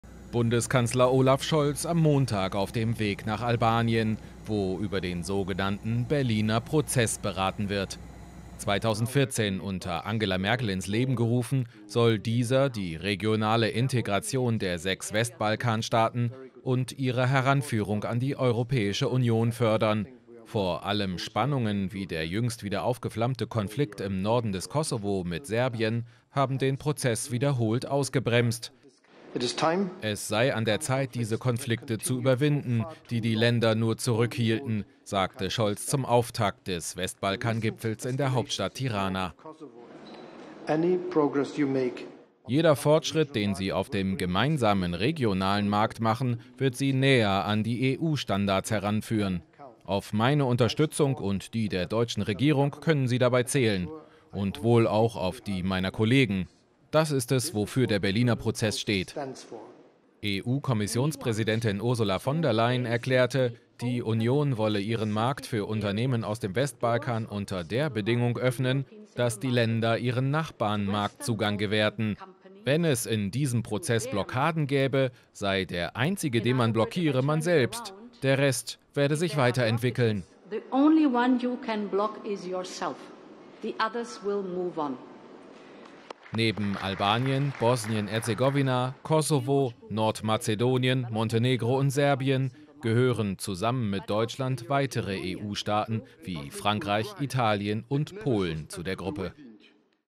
Scholz bei Westbalkan-Gipfel: „An der Zeit, lokale Konflikte zu überwinden“
Spannungen zwischen dem Kosovo und Serbien hatten den Berliner Prozess zuletzt ausgebremst. Es brauche zunächst wirtschaftliche Annäherung zwischen den Westbalkan-Staaten, damit diese EU-Standards erreichen könnten, mahnt Scholz zum Gipfelauftakt in Tirana.